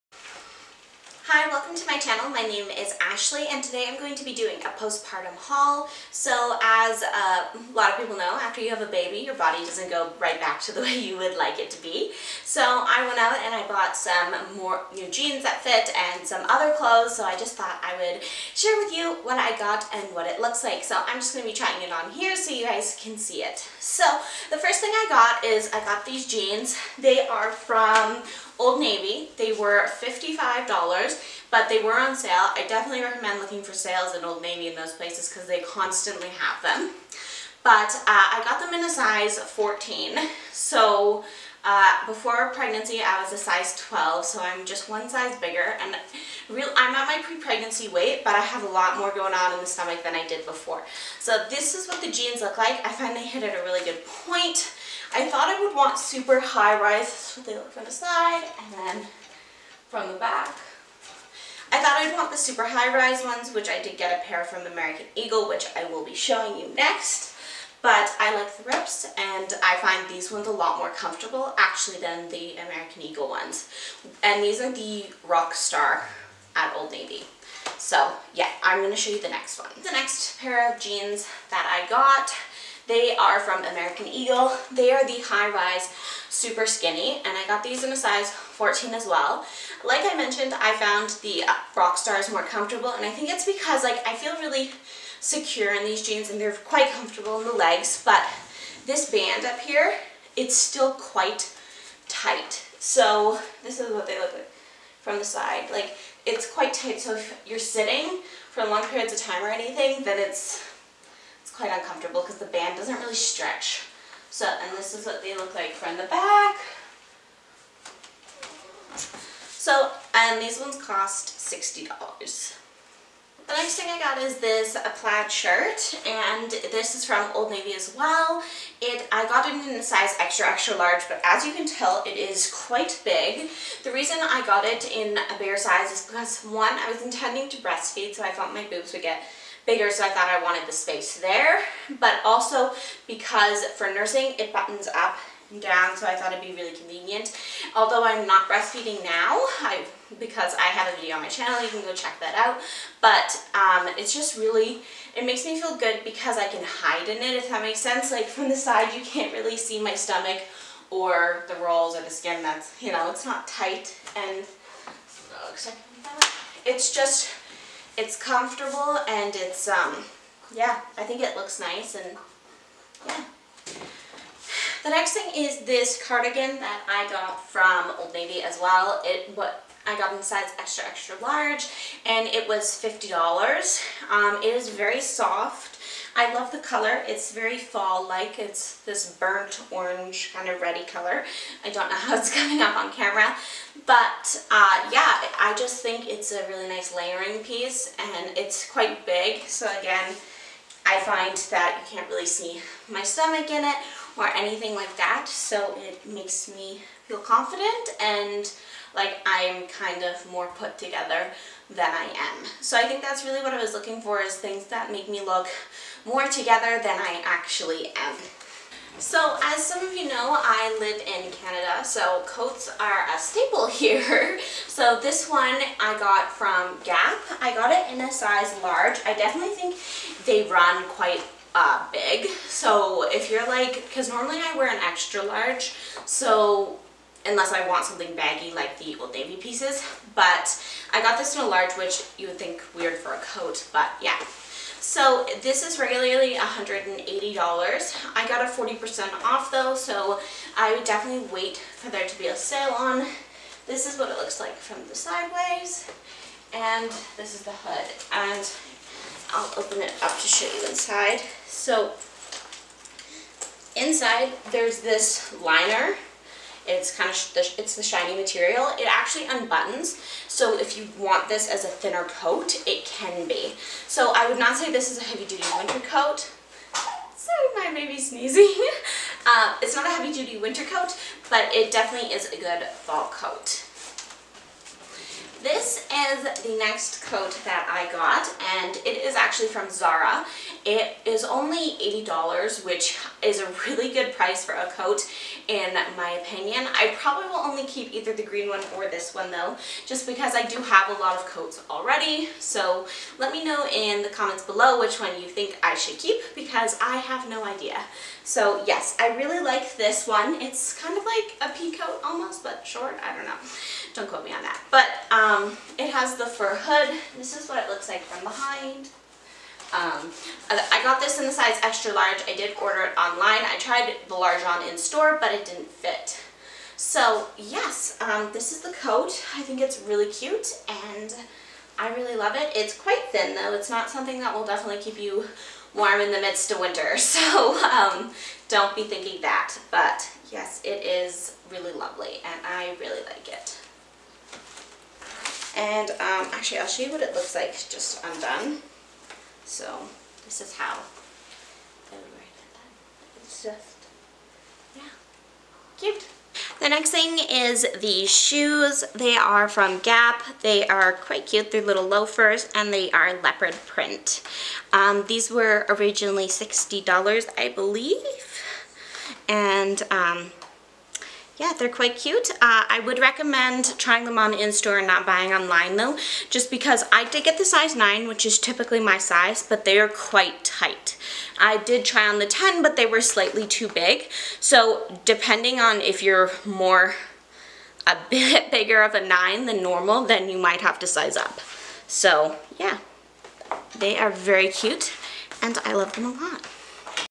Post baby body haul
Hi, my name is Ashley and I have two identical twin daughters! IN this video I show you clothing I have recently purchased to make me feel more confident in my post baby body.